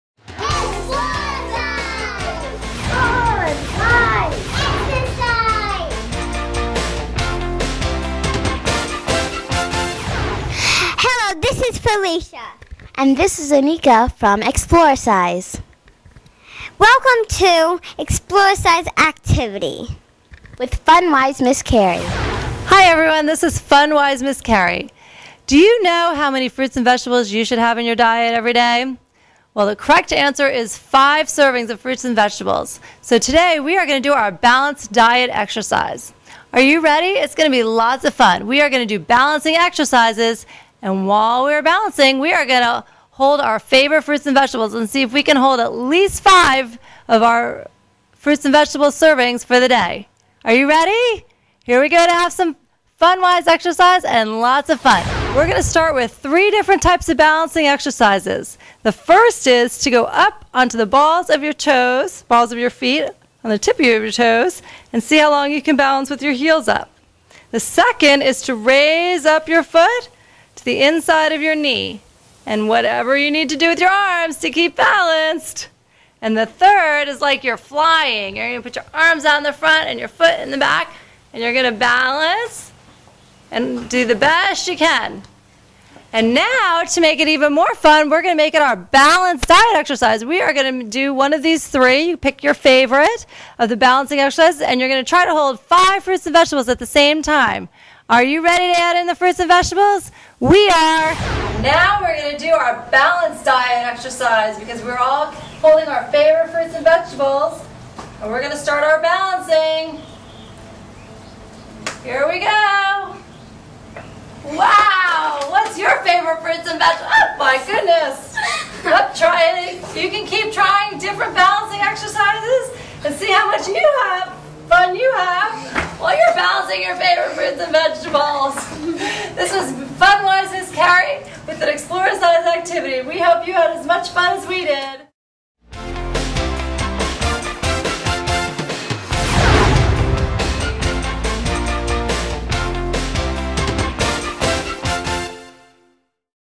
Balance Diet Exercise
Fun Wise Ms Carrie joins Exploracise stars Aaron, Felicia, and friends for a balanced diet exercise.  Statistics show that children learn best while having fun.  for more ways to incorporate fun and play into your child's academic life.